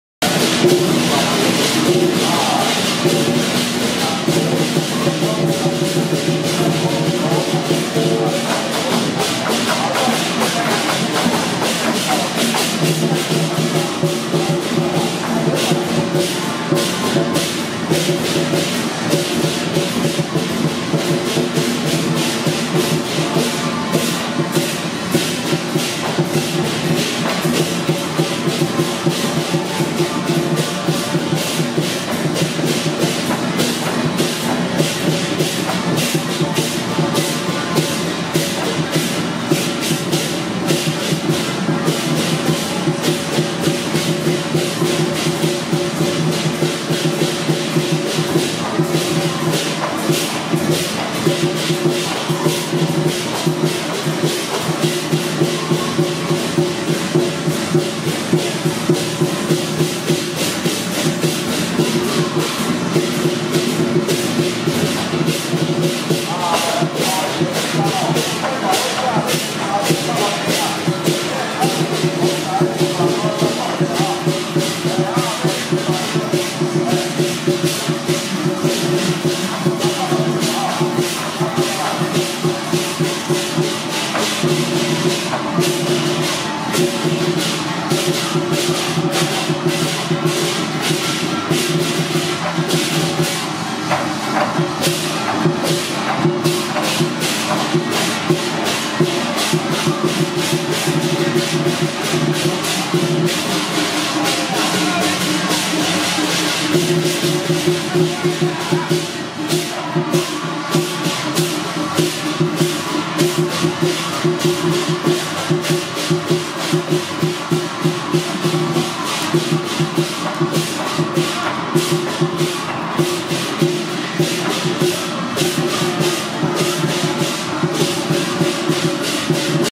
105.5.22南鯤鯓代天府李府、范府千歲進香期~【乩童退駕拔口針】
2016/5/22南鯤鯓代天府李府、范府千歲進香期